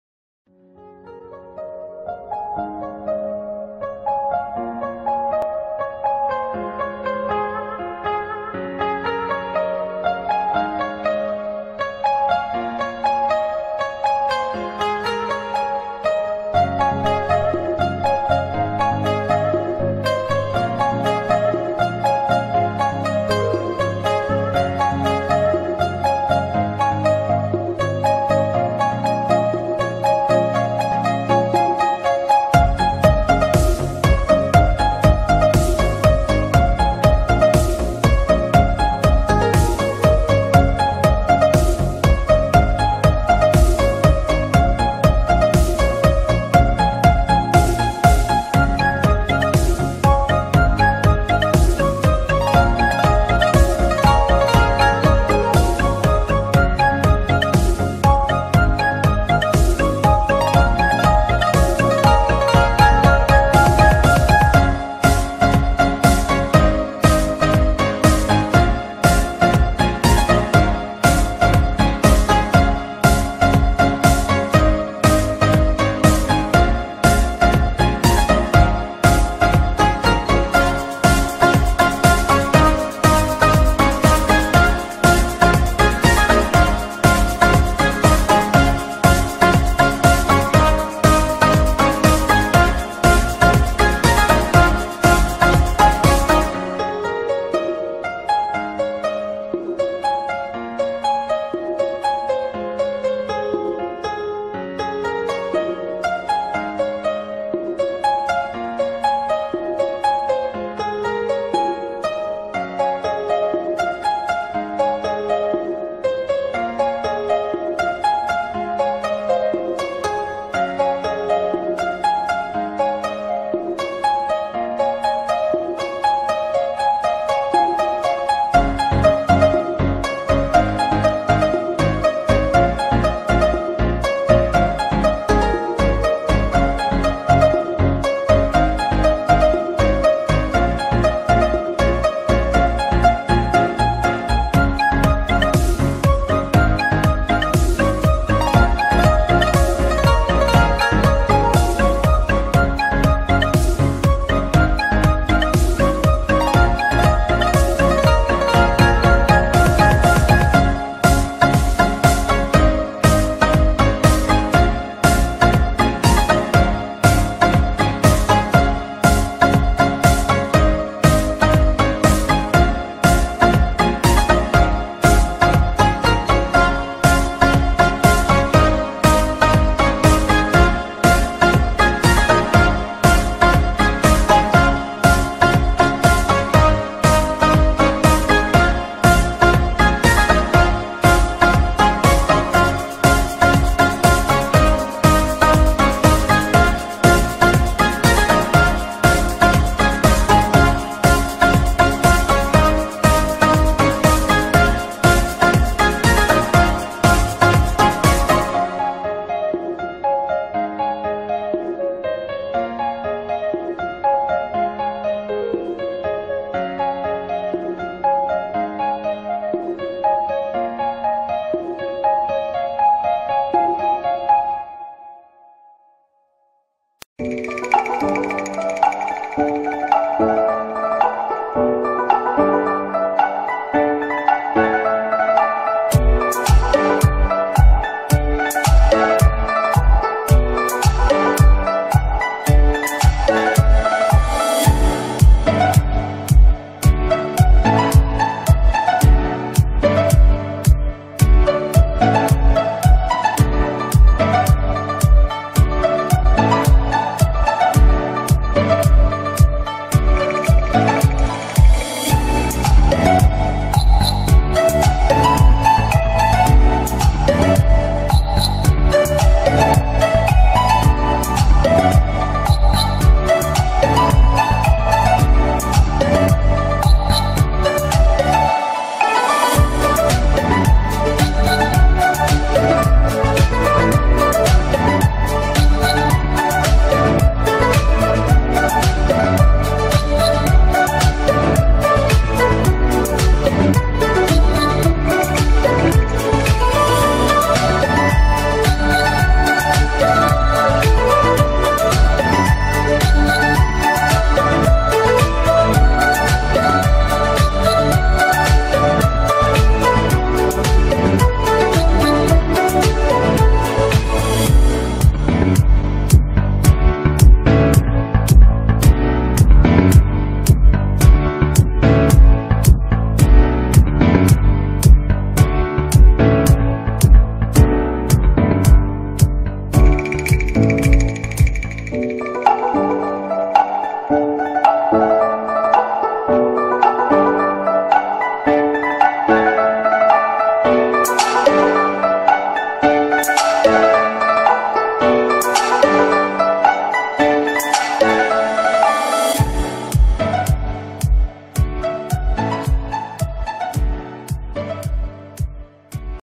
Thủ đoạn cũ nhưng mà rất mới với những người chưa biết ! nạn nhân bị mất IPhone X, một số tiền ...
Quốc Dự_Digital.